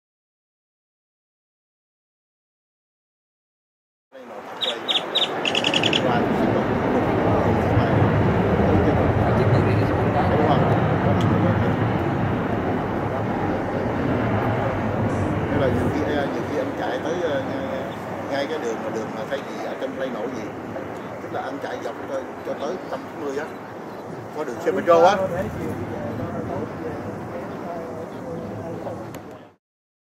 Air China 777-39L/ER [B-2086]
Air China 777-39L/ER en-route from ZBAA to KIAH, over Dallas/Fort Worth International Airport (DFW) on June 08, 2014